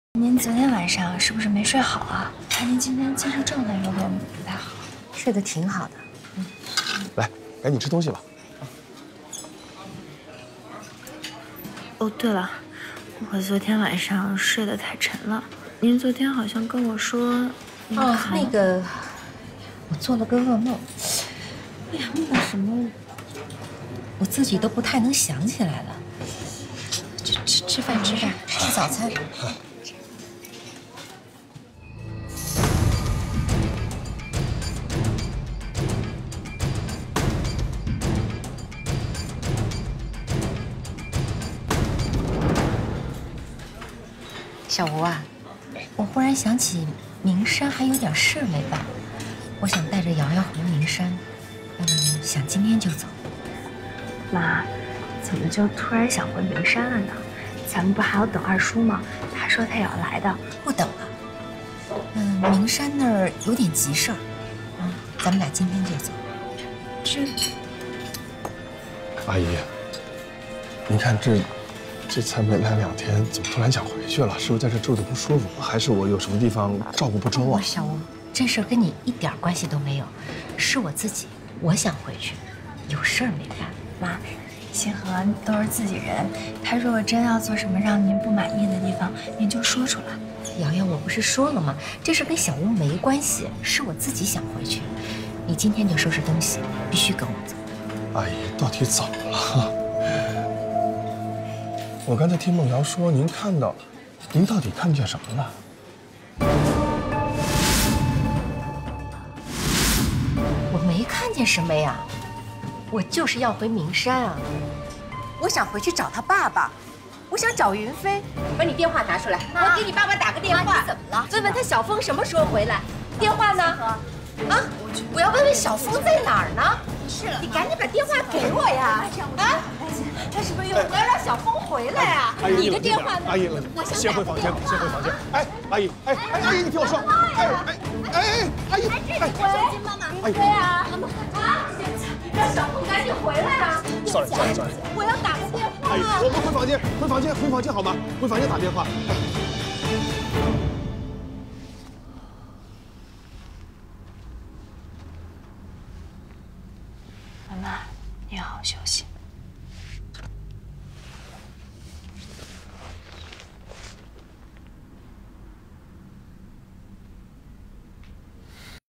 李玫为了逃离吴新河的控制，假装病情复发，想要回明山
因为片方版权限制，目前台湾、马来西亚、文莱这些地方暂时不可看哦~
该剧讲述了高智商、高学历的化学工程师吕云鹏，因为亲情触发，秘密潜伏进入金三角势力庞大的毒枭集团，与毒贩展开斗智斗勇经历九死一生，最终成为一名功勋卧底的故事。